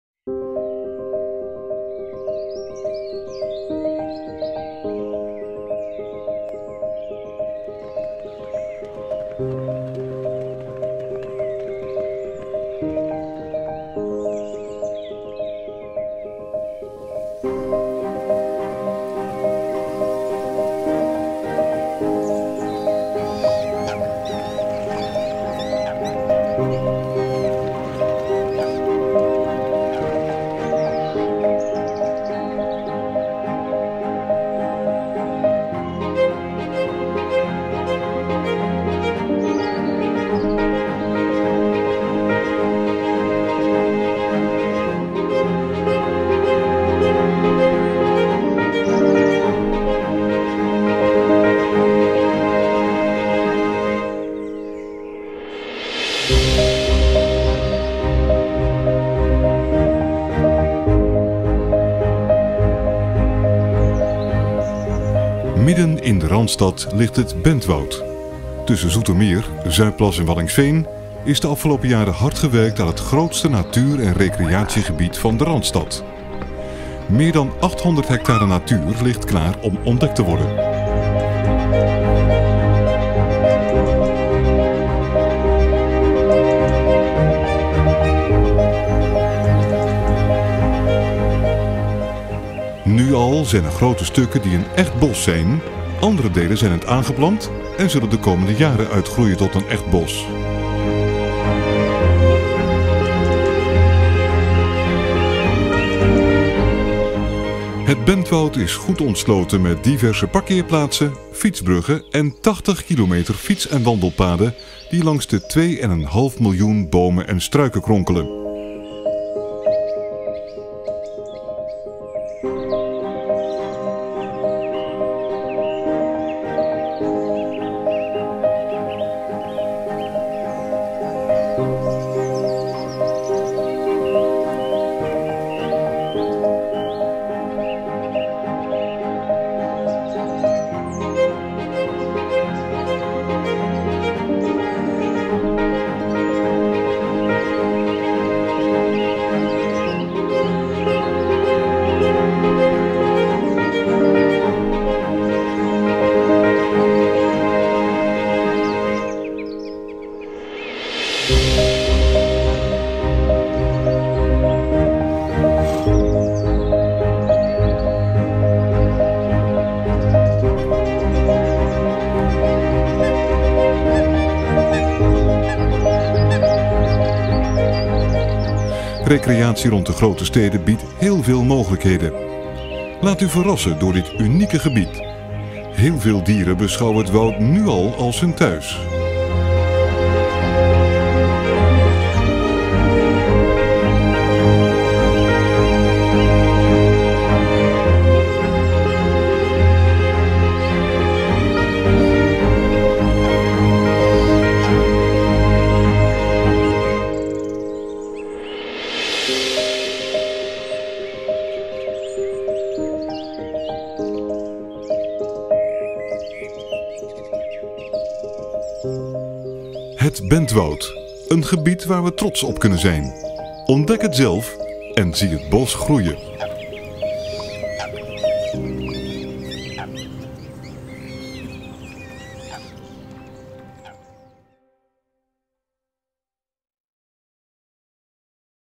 Het Bentwoud
During the spring of 2016 Hollands newest woods a.k.a. "het bentwoud" was starting to reach its full form. As part of the celebrations in June we shot this short nature doc about its flora and fauna.